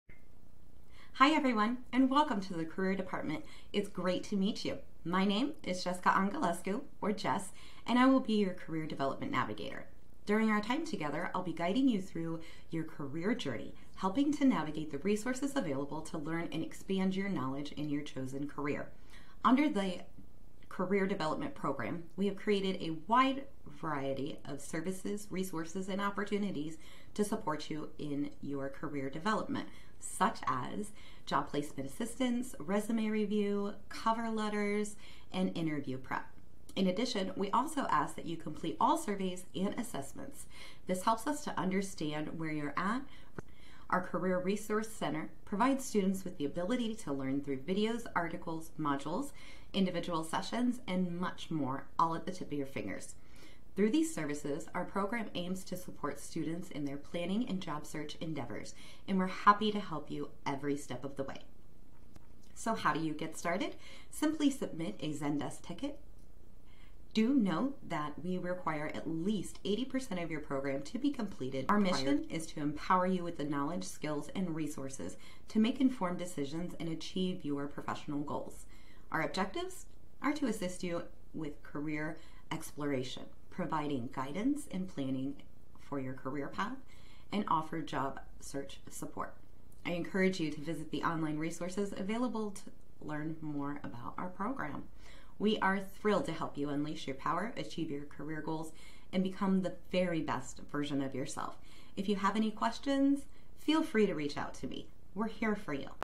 ACT's Career Planning and Professional Development Center
In this video, ACT's career coach takes you on a virtual journey through our Career and Professional Development Center. Designed specifically for our students, this video is your gateway to unlocking a world of opportunities.